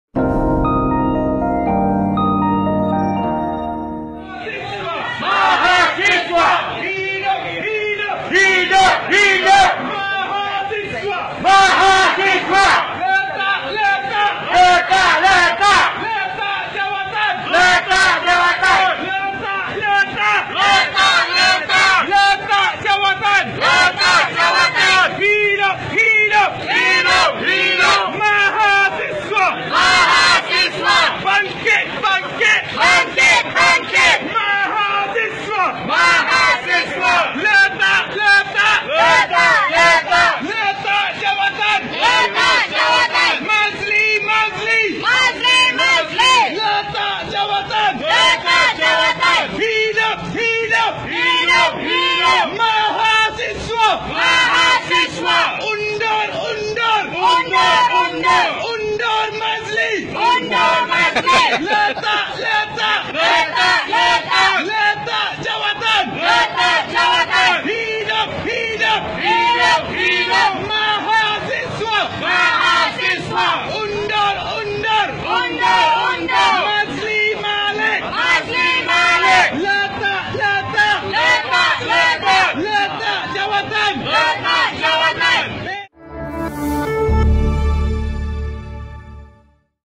Maszlee Gelakkan Mahasiswa Konon Nak Tunjuk Perasaan
SEMUA sedia maklum,  Menteri Pendidikan, Dr. Maszlee Malik sah dilantik sebagai Presiden Universiti Islam Antarabangsa Malaysia (UIAM) yang ketujuh selama tiga tahun. Hal ini menimbulkan rasa tidak puas hati sekumpulan pelajar serta aktivis yang menggelar diri mereka Gabungan Pembebasan Akademik IPT.

Mereka juga telah mengadakan tunjuk perasaan bantah duduk, bersama tidak lebih 10 pelajar dari beberapa universiti, di pejabat Kementerian Pendidikan sejak jam 8 pagi pada Jumaat dan Sabtu lalu. Namun, reaksi Menteri Pendidikan itu menarik perhatian ramai. Lawak pun ada.

Layari straight to the point!